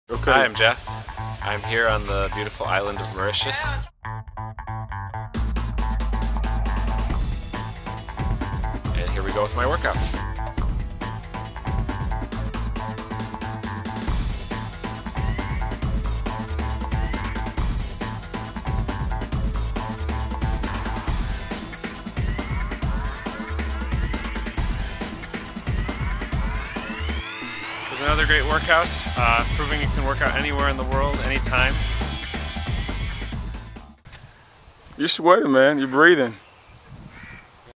inerTRAIN Workout - Mauritius
inerTRAIN's CEO, Jeff, proves that you can take your personal trainer with you and get a great workout in anytime, anywhere! His customized 10-30 minute on-the-go workouts are developed by his trainer, Andrew, based on where he's able to work out and how much time he has on a given day anywhere in the world! Best of all, they are all aligned with his goals to get the most out of his limited time.

With inerTRAIN, Jeff is truly going around the world with his custom workouts!!

This workout is done at a beach resort on the beautiful island of Mauritius - taking advantage of the weather, the beach, the incredible scenery, and the naturally occurring "fitness equipment". If you can't get to a gym, your trainer will get the gym to you!